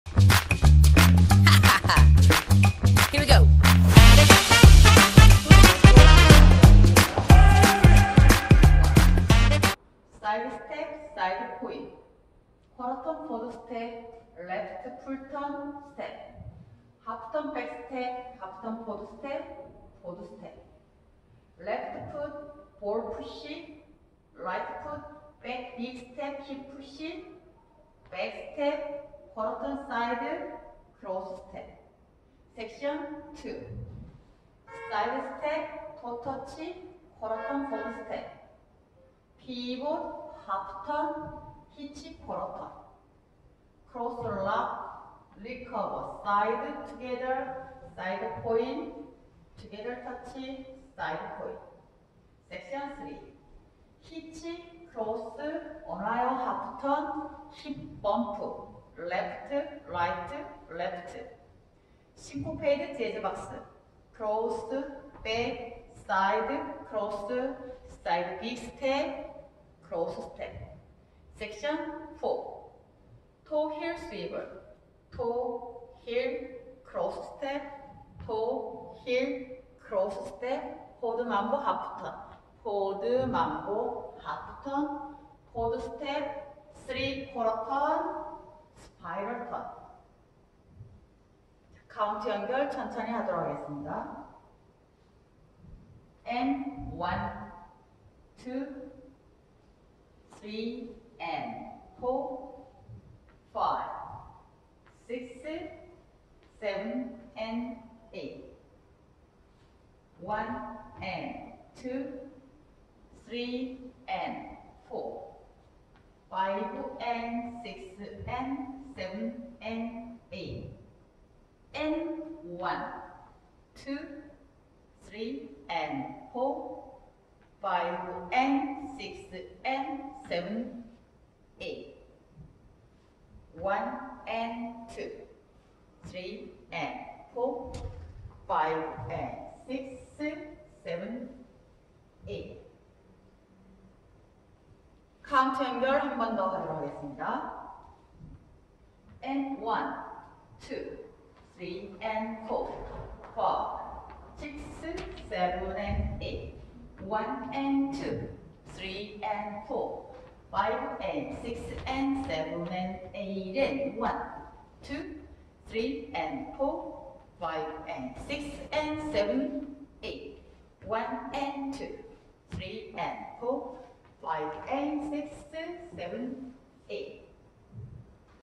◆◆ Teaching / Good To Go / KoSCAA코스카부산1지부
황은정라인댄스 KoSCAA # 한국스포츠문화예술협회
Choreographed by : Scott Blevins (Feb 2021)
32 count - 4 wall - Intermediate level line dance
★ Sequence : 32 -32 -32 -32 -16 count - 24 count - 32 -15 with ending